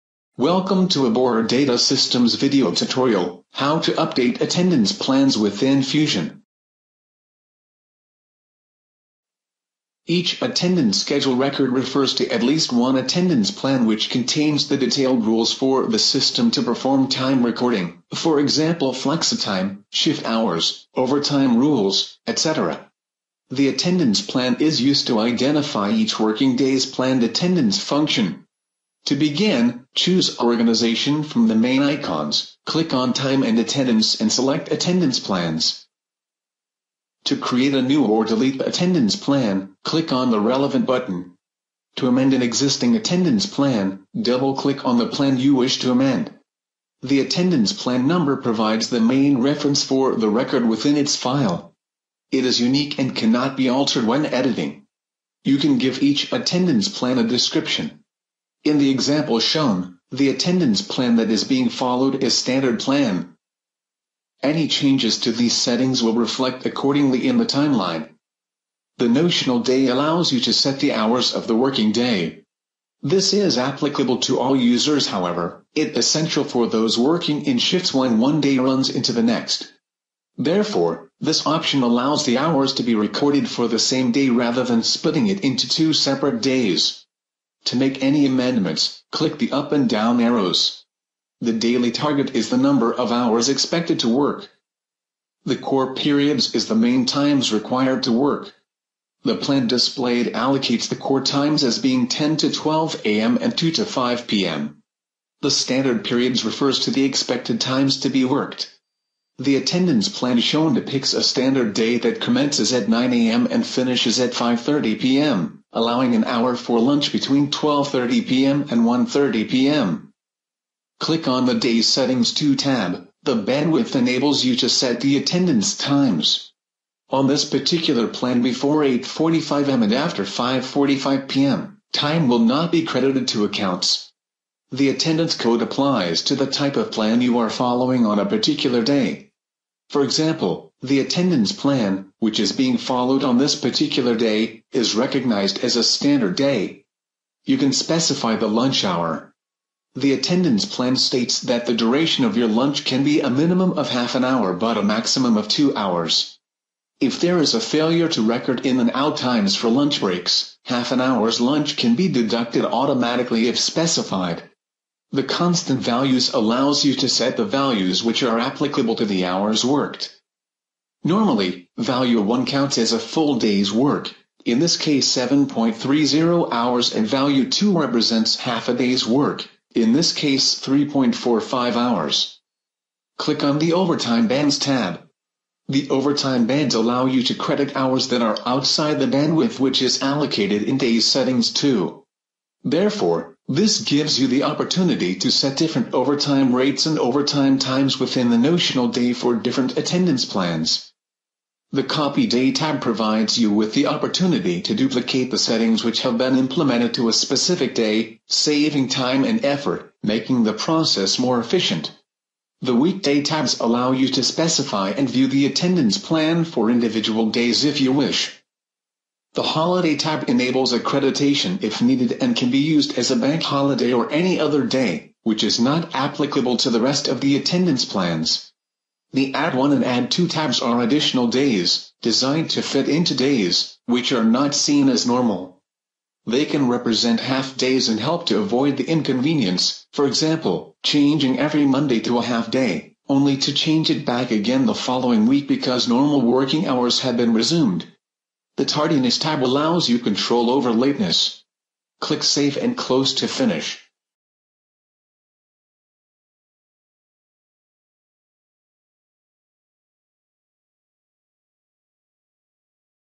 Borer FUSION T&A - Attendance Plans
Borer's FUSION software integrates Access Control, Time & Attendance, ID Badge Design & Production and Visitor Management into one package.

All videos are presented with English subtitles and commentary.

This video will demonstrate how to update attendance plans within FUSION.

Each Attendance Schedule record refers to at least one attendance plan which contains the detailed rules for the system to perform time recording, e.g flexitime, shift hours, overtime rules, etc. The attendance plan is used to identify each working day's planned attendance function.